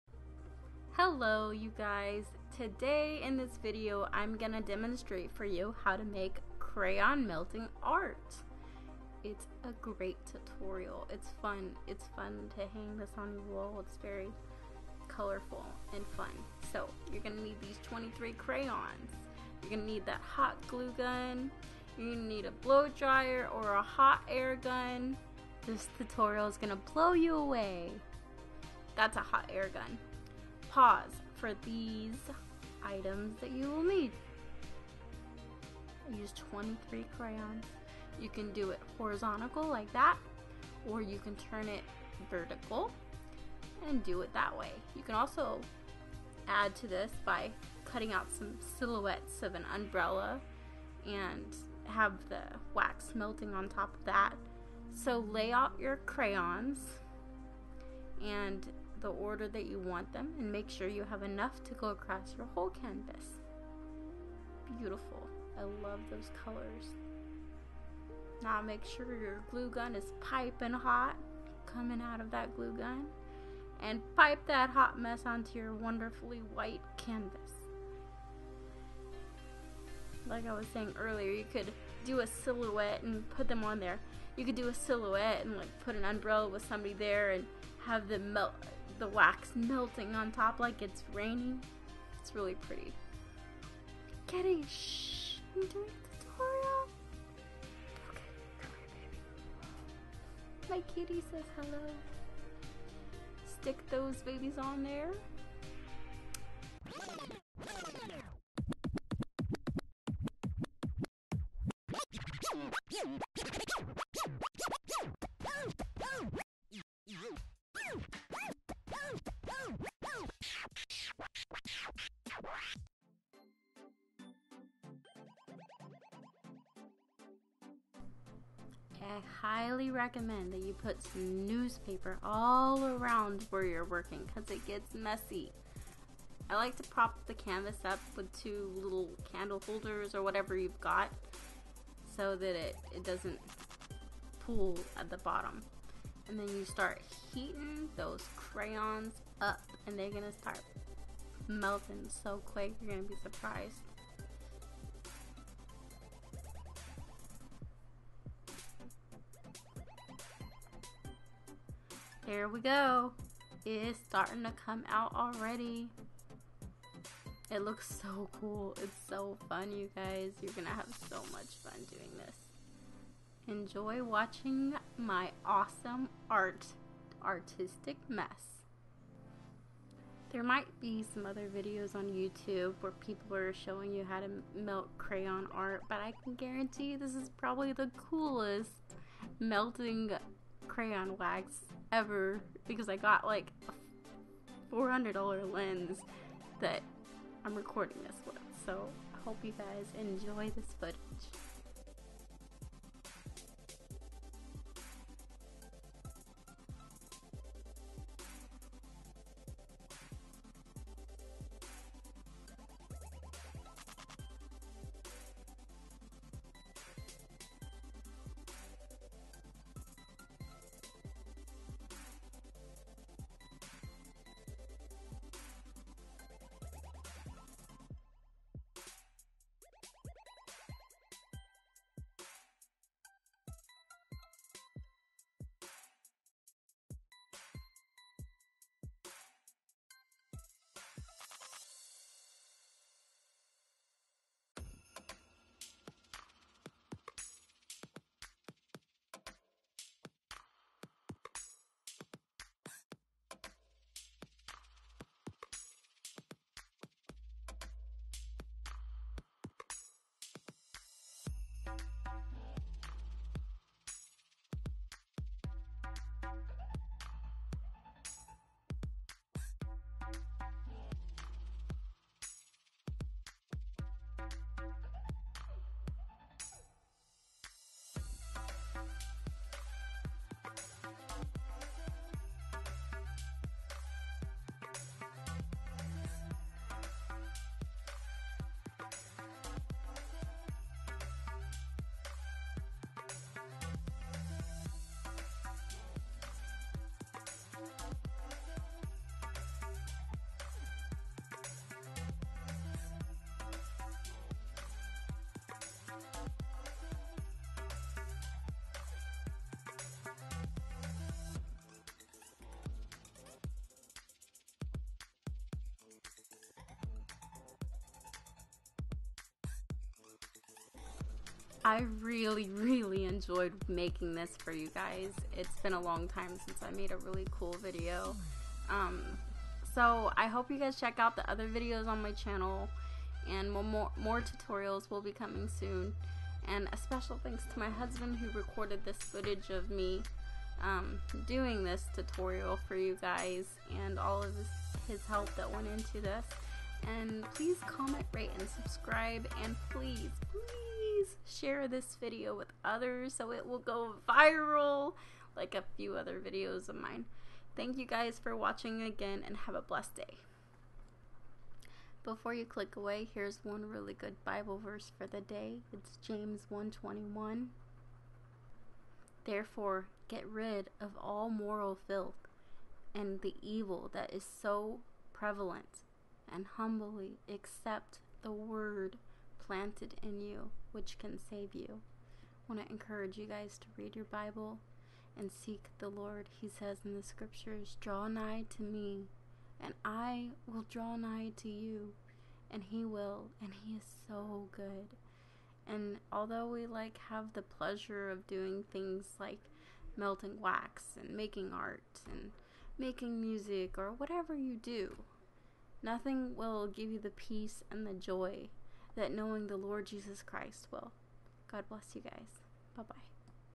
Crayon Melting Art Tutorial
Tutorial crayon melting art on canvas for home decorating.
Fun and easy DIY kids art and craft project. This was so fun, easy and cheap to make. I encourage you to give it a try! Encouragement would be great to get me to make more videos in the future.

List of things you need
23 Crayons of your color choice
or
Hot Air Gun
Newspaper for easy cleanup!